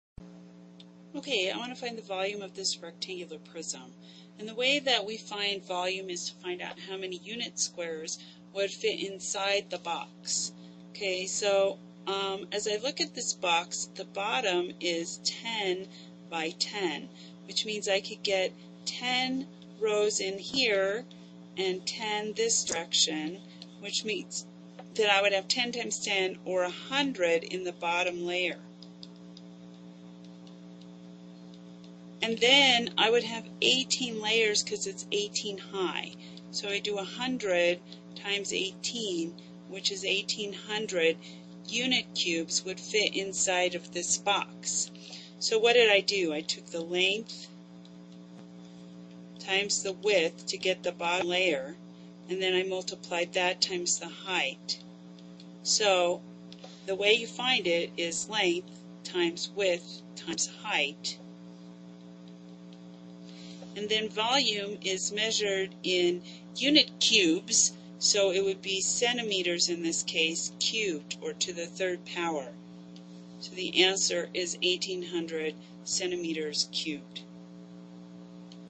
Volume of Rectangular Prism
How to find the volume of a rectangular prism